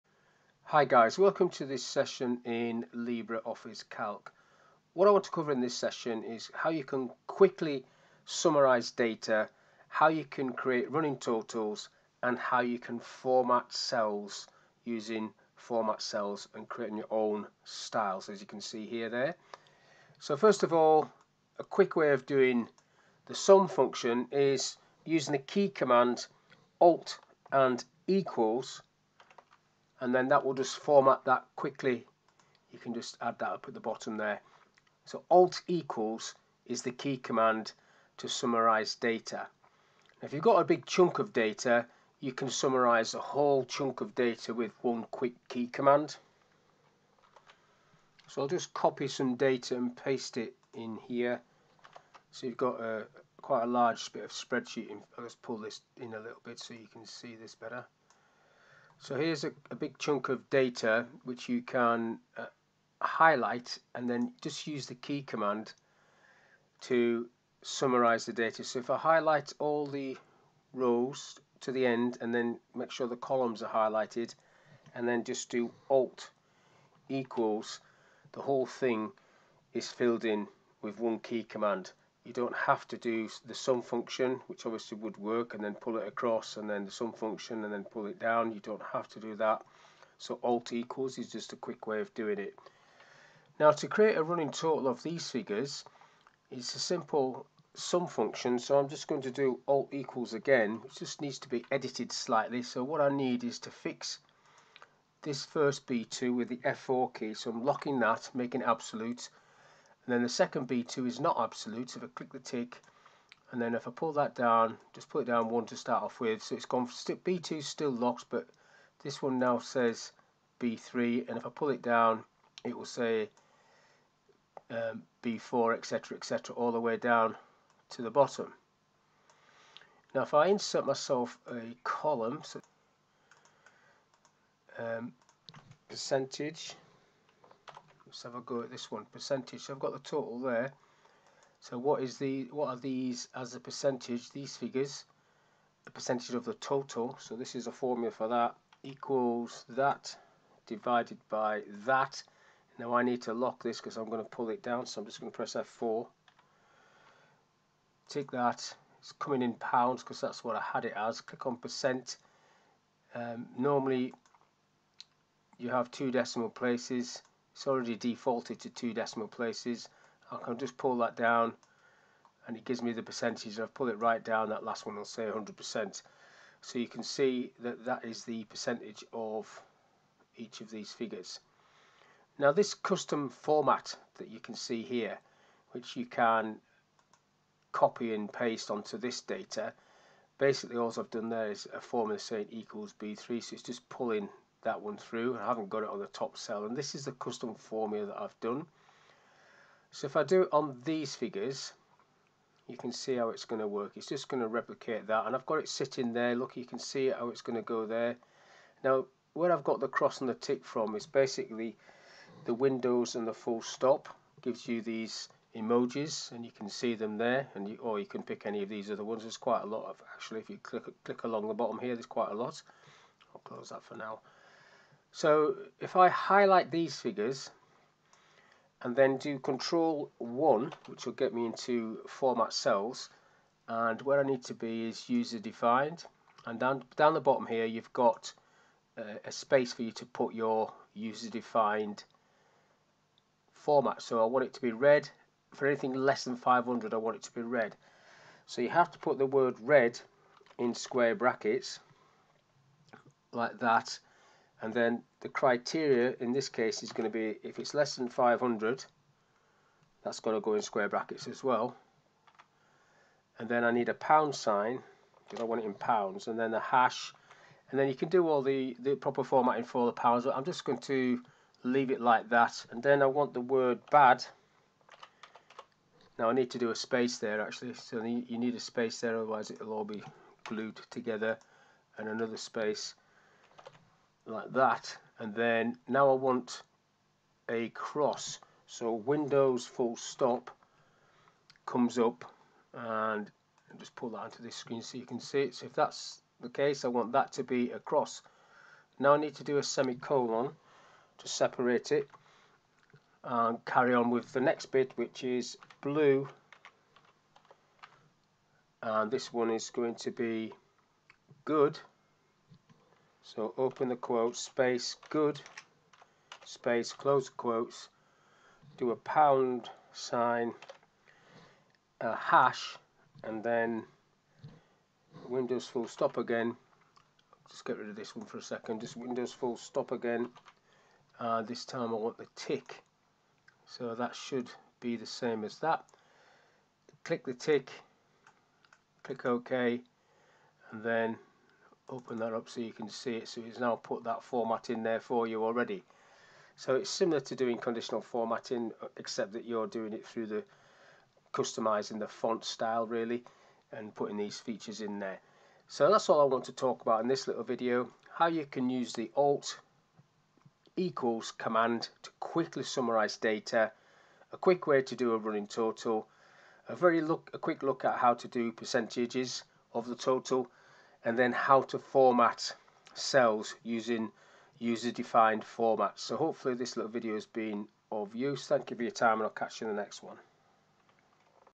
Quick way to summarise data, create percentages and customise cells
This is a LibreOffice Calc tutorial covering hot tips to summarise data, create percentages and develop your own cool custom formats.  The tutor starts off by using the ALT key and the equals sign to summarise data.  the tutor then uses the F4 function key to lock cells and create percentages.  The finalpart of the viudeo coves how to cr4eat your own super cool formats using Windows icons. Quick way to summarise data, create percentages and customise cells. Use Windows icons.